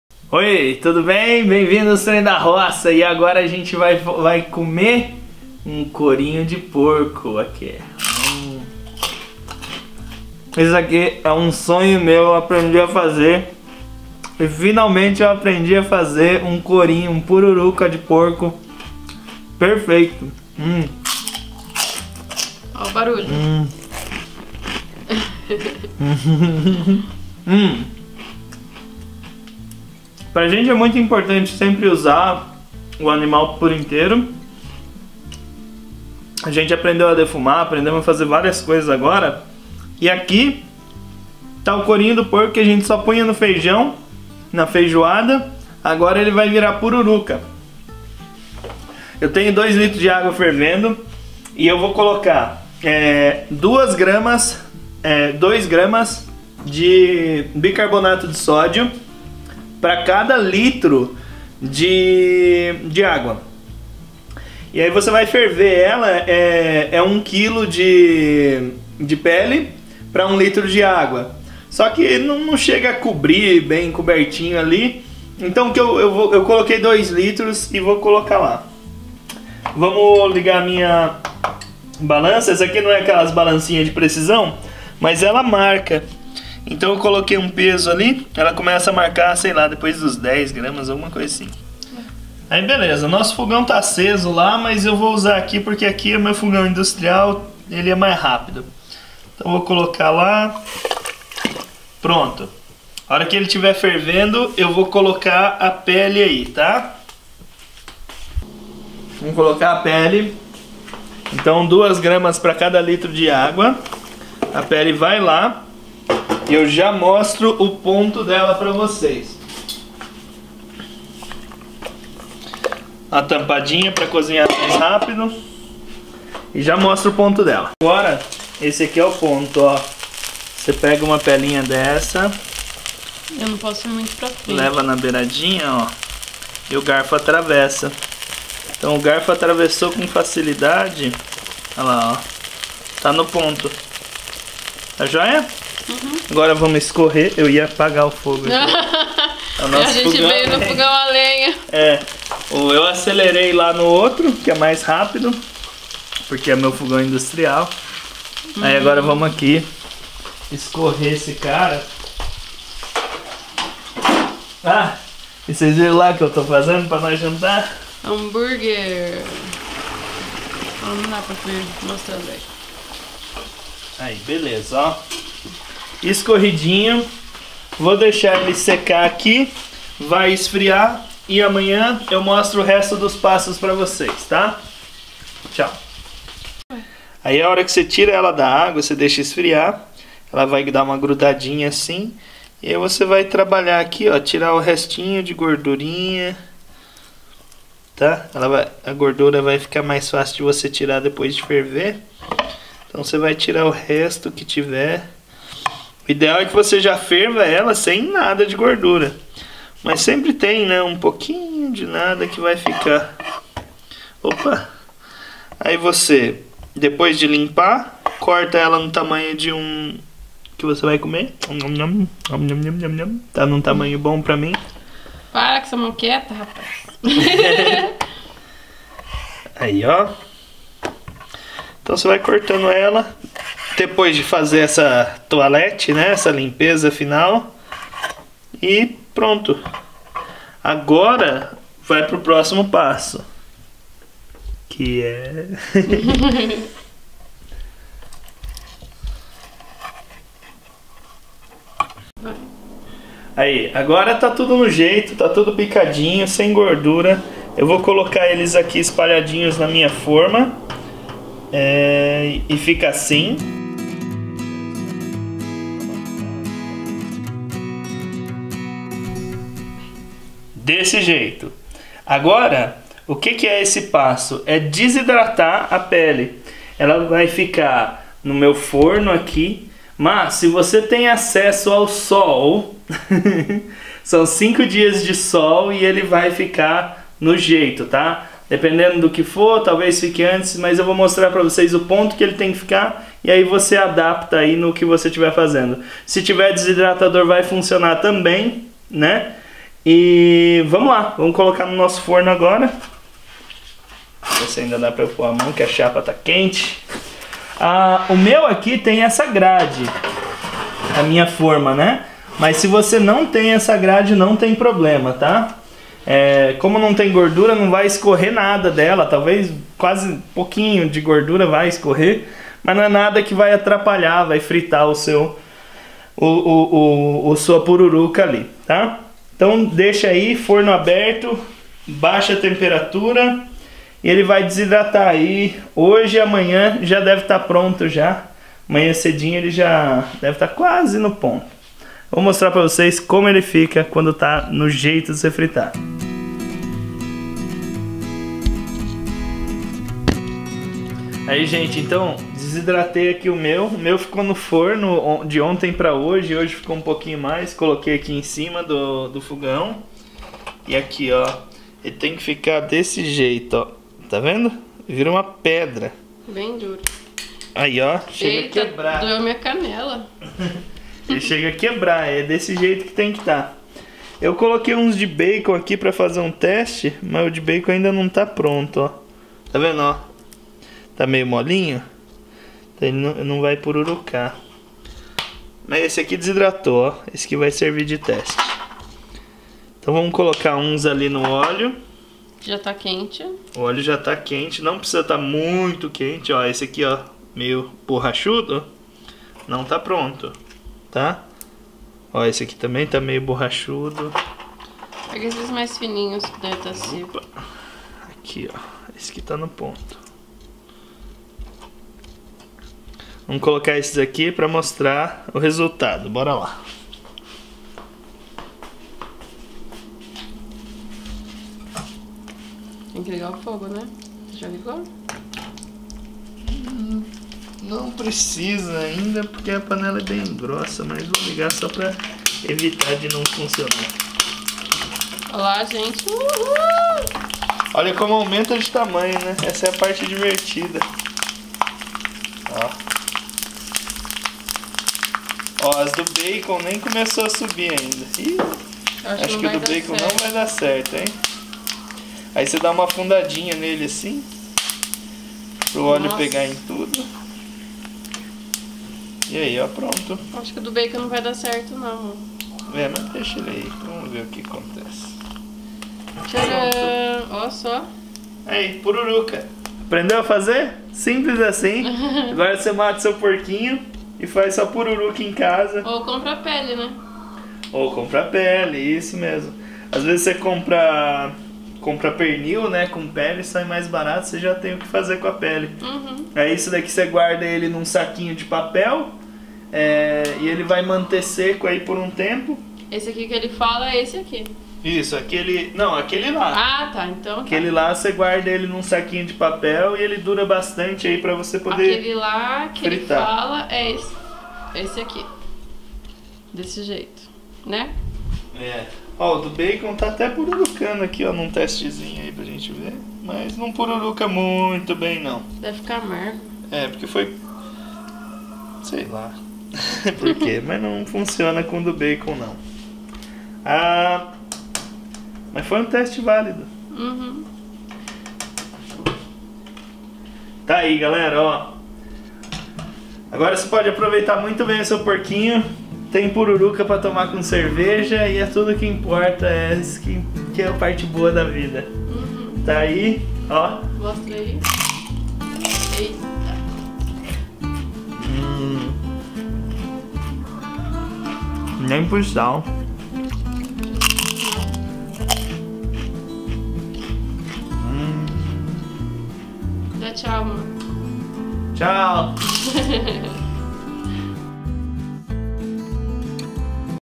A PURURUCA PERFEITA - Como faz o courinho de porco a pururuca perfeito
A PURURUCA PERFEITA
Como faz o courinho de porco a pururuca perfeito.
Sabe aquela pele de porco que você ía jogar fora, ou colocar no feijão?

Então! Vamos fazer uma pupuruca perfeita?
Sequinha, gostosa, saborosa, que come tomando aquela cerveja bem gelada?
Elton demorou anos pra aprender, e vai te dar a receita aqui nesse vídeo.
VAMOS APRENDER?

Para nos enviar uma cartinha:
Caixa Postal 10 - cep: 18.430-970

ou  @ostremdaroca no app.

Bóra assistir esse vídeo?
Gratos por você aqui!